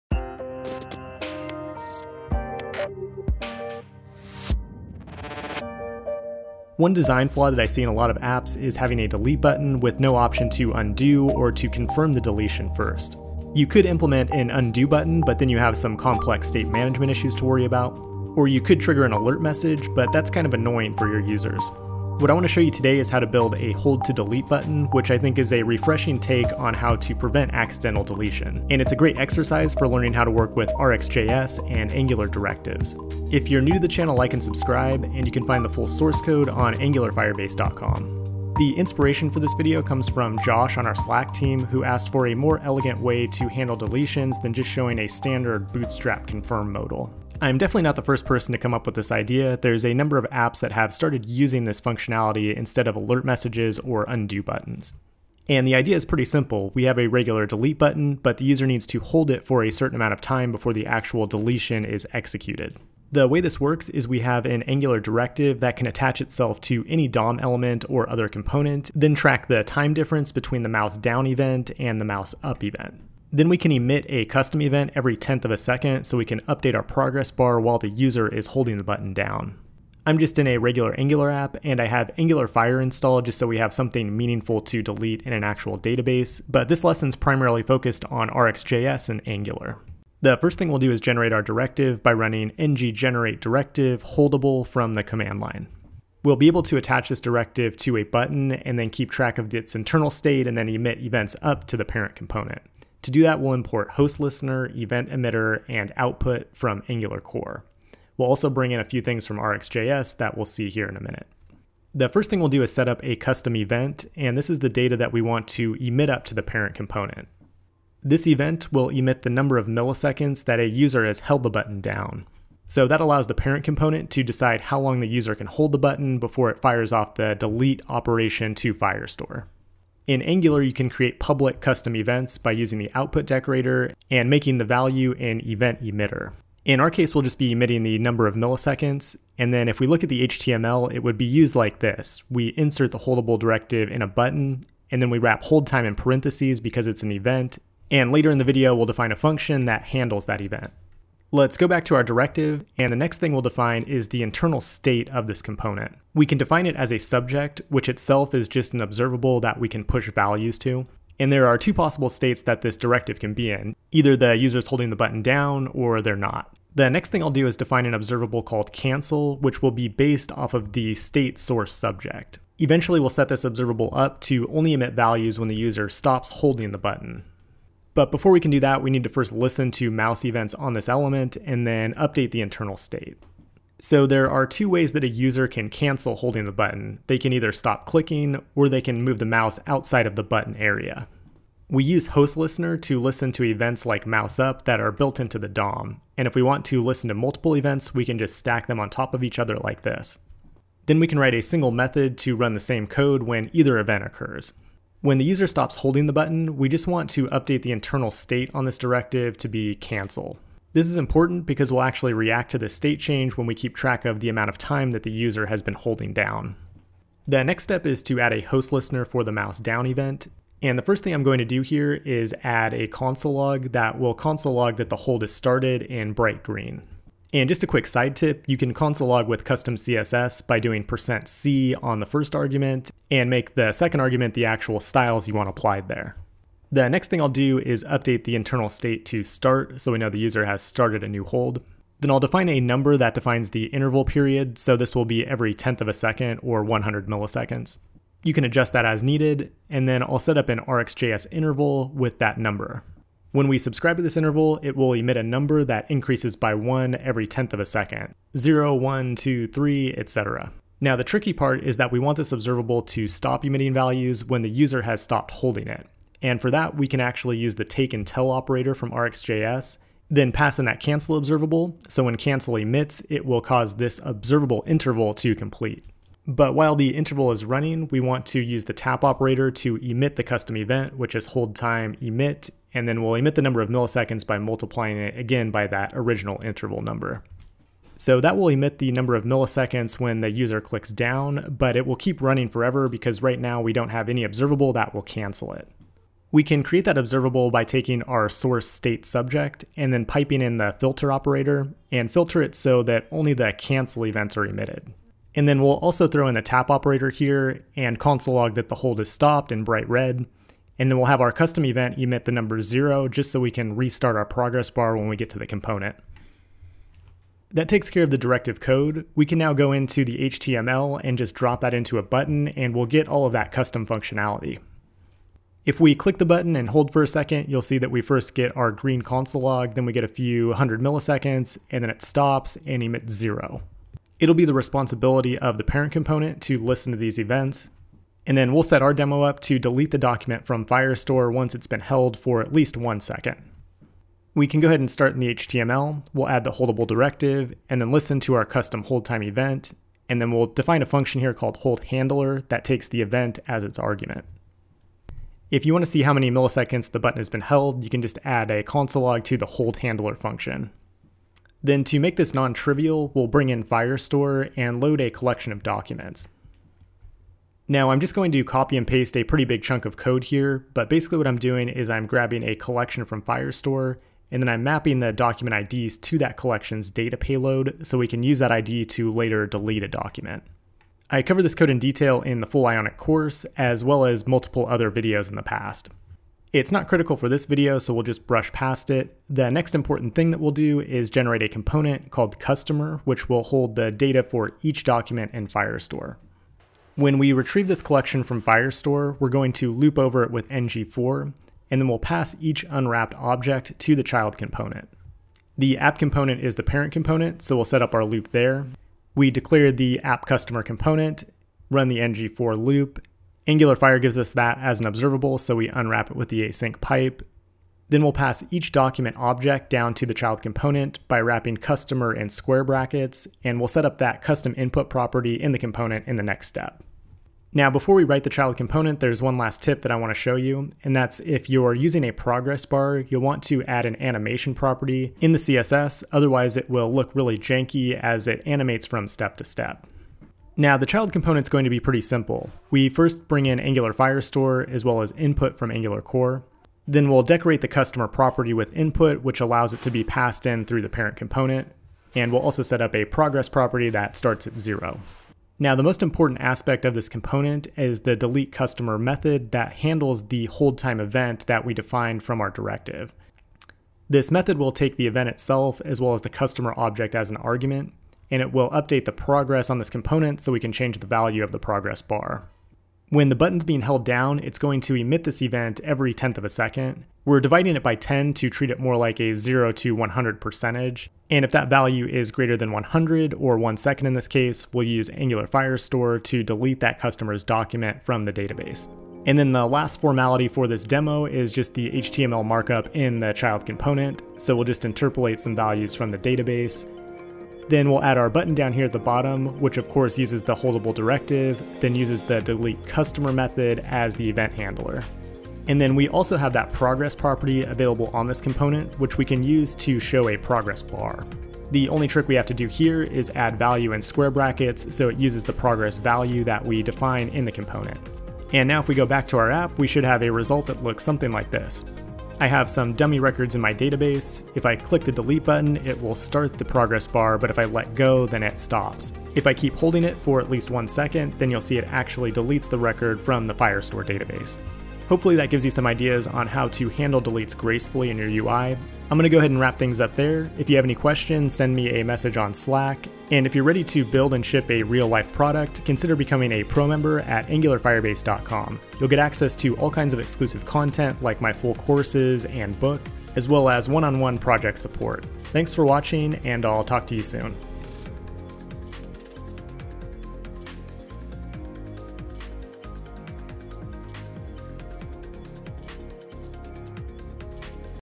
Angular Directives - Build a Hold-to-Delete Button
Learn how to use Angular Directives to build a hold-to-delete style button that removes a document from Firestore after holding down for 1 second.